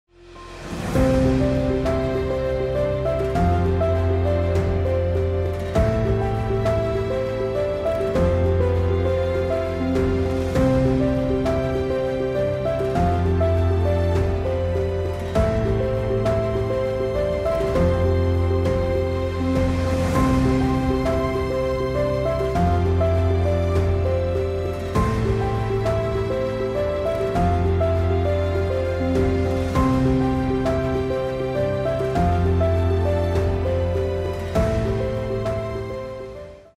JeetengeOlympics | 54 Days to Tokyo Olympics
Sutirtha Mukherjee on representing India

Sutirtha Mukherjee is working hard and proud to represent India in her first Olympics. Join us in wishing her good luck for TokyoOlympics